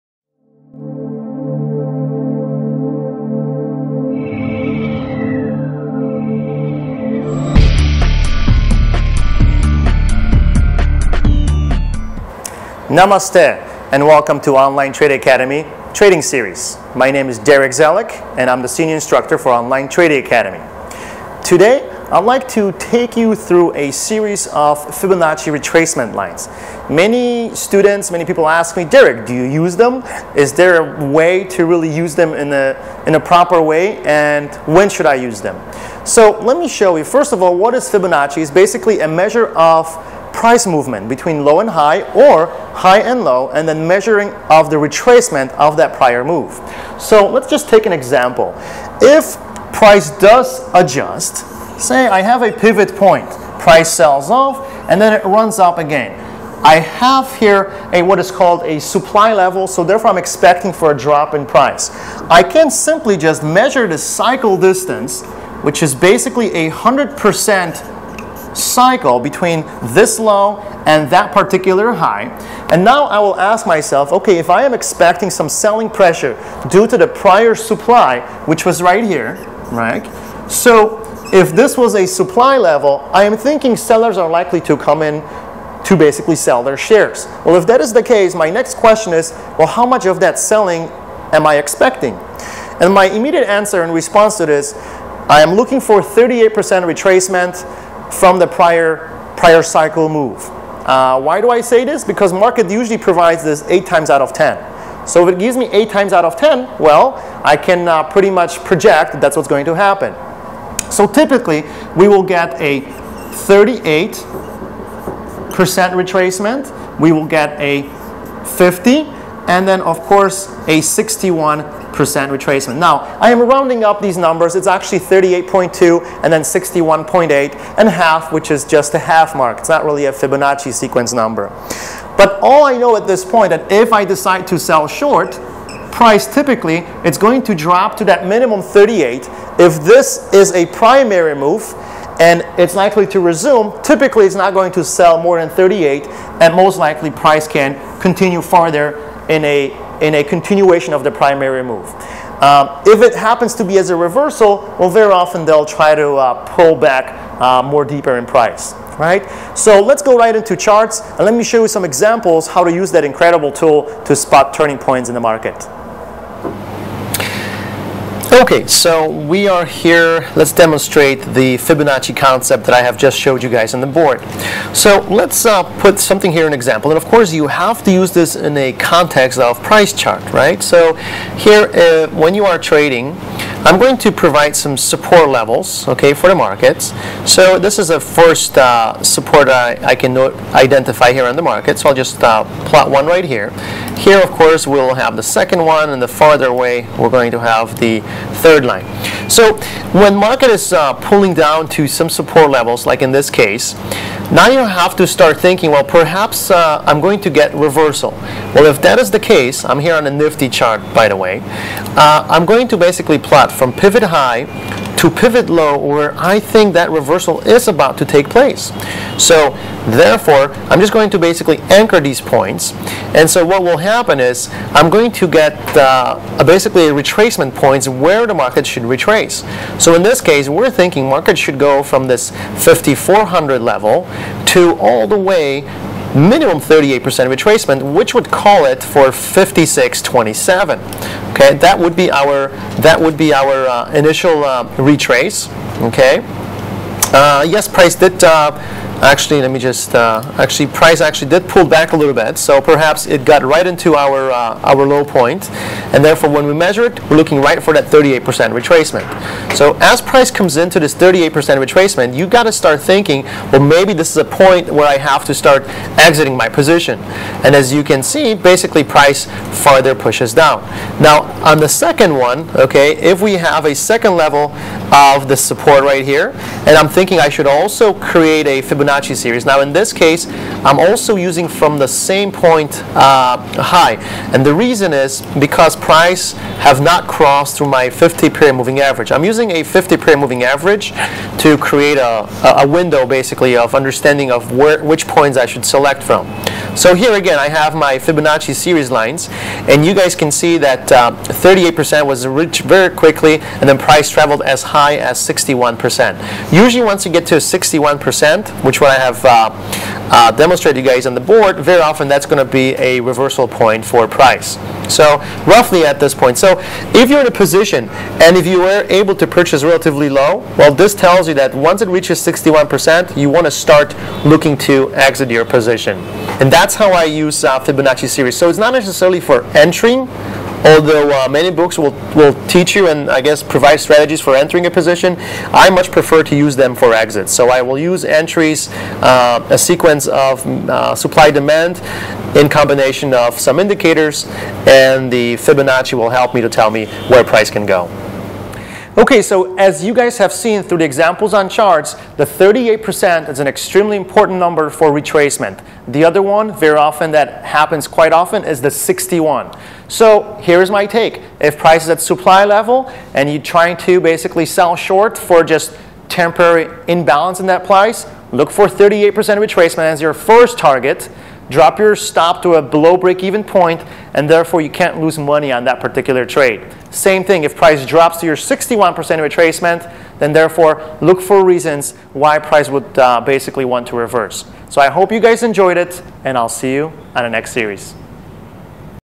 Understanding Fibonacci Retracement Lines by Sharekhan
Understand Fibonacchi retracement lines with Online Trading Academy's instructor Darek Zelek.